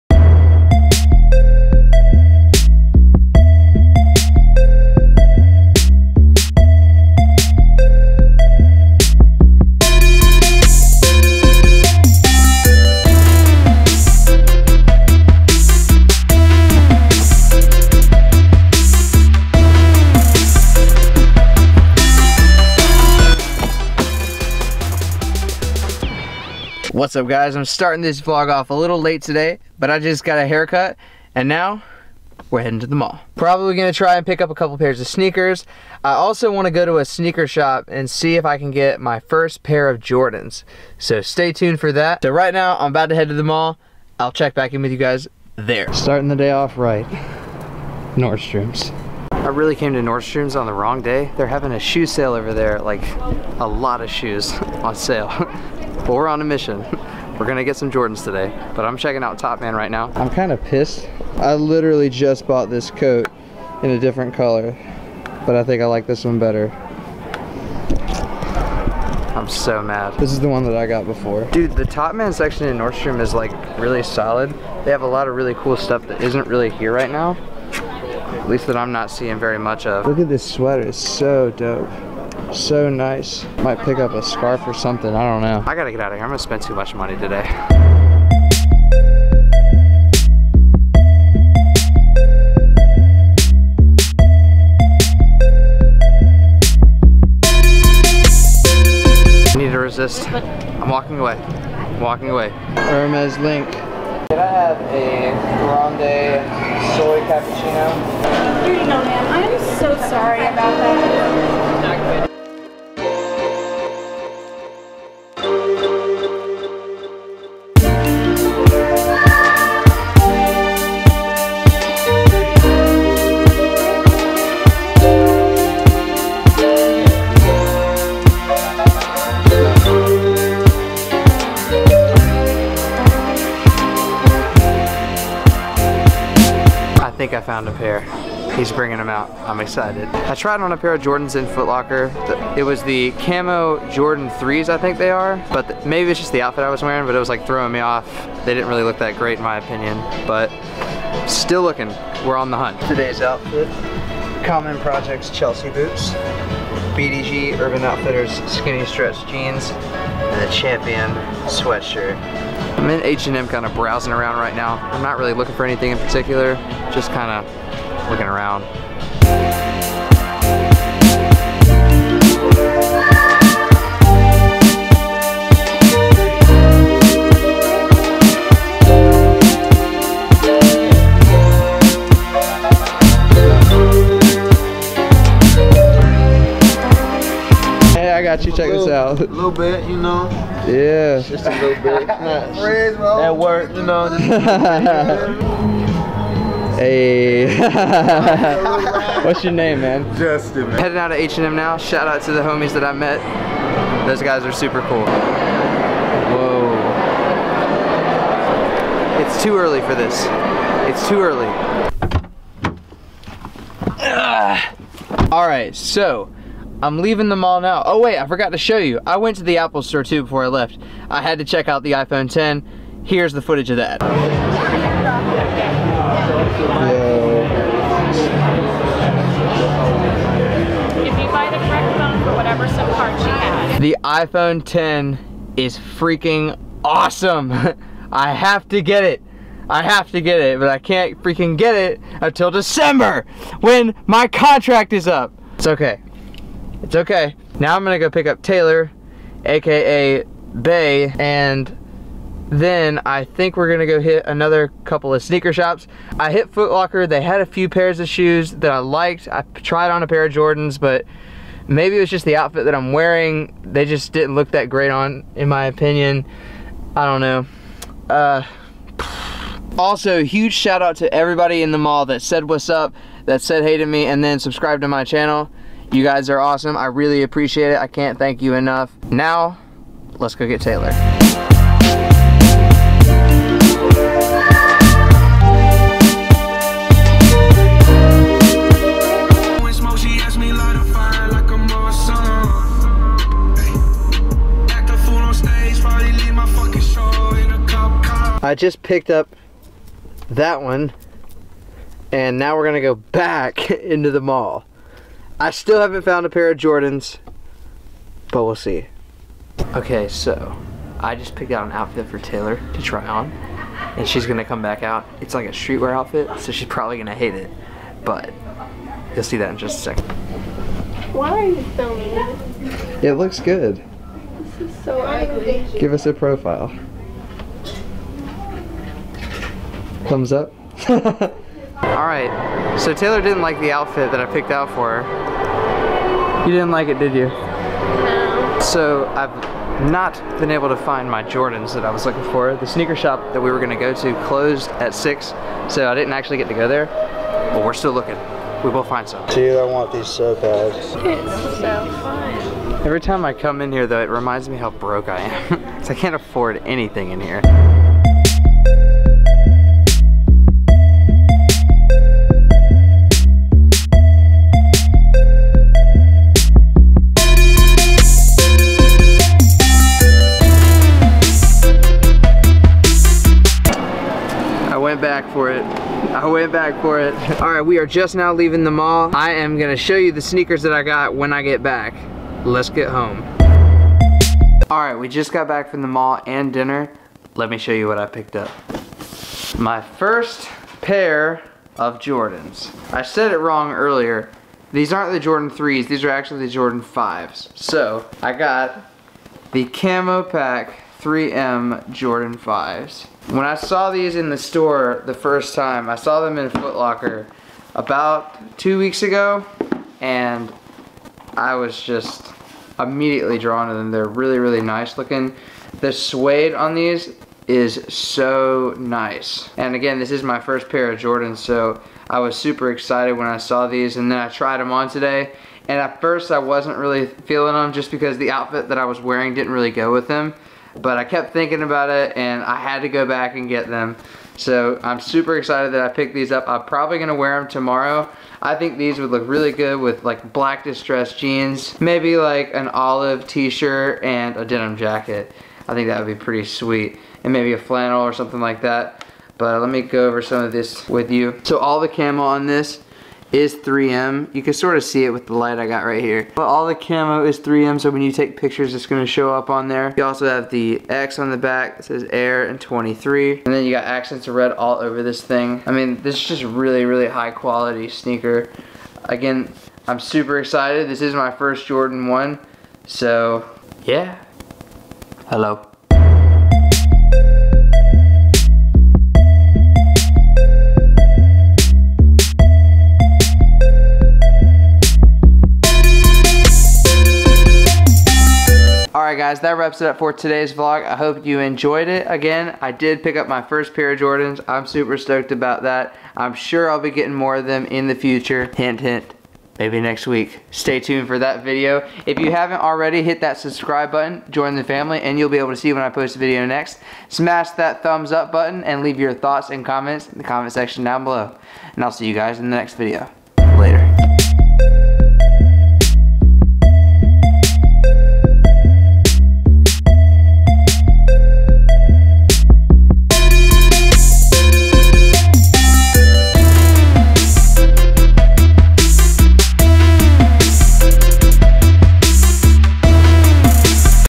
MY FIRST PAIR OF JORDANS! | VLOG #5, Jordan V, Champion, Common Projects, Men's fashion VLOG
What's up EVERYBODY!!! Thank you for tuning in!!!
In this episode, VLOG #5, I take YOU with me on the HUNT for MY 1st Pair of JORDAN'S!

Camera: Canon 80D
Lens: Canon EF-S 10-18mm f/4.5-5.6 IS STM Lens
Microphone: Rode VMGO Video Mic GO
Gorilla Tripod